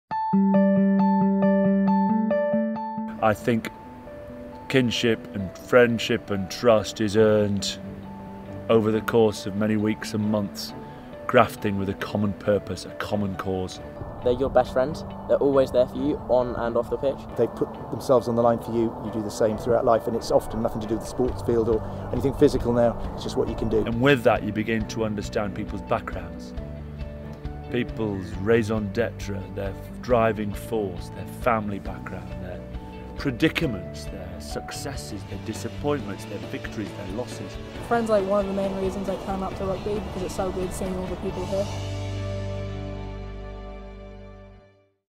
Will Greenwood - The importance of Kinship
Will Greenwood discusses with AEG the importance of kinship and how trust is built within a team.